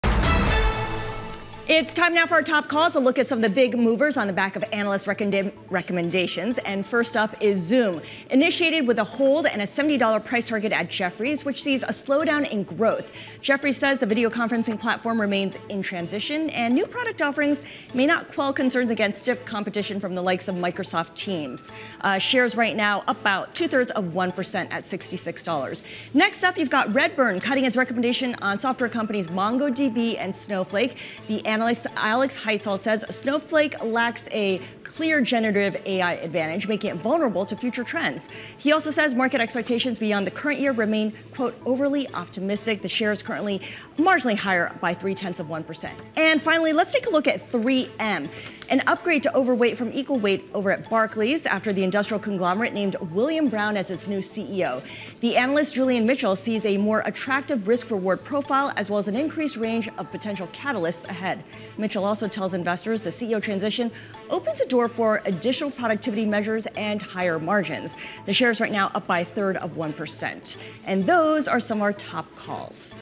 Zoom Growth Slowdown, Snowflake Downgraded, 3M Upgraded with New CEO | Top Calls
Top calls today are Zoom Growth Slowdown, Snowflake Downgraded, 3M Upgraded with New CEO.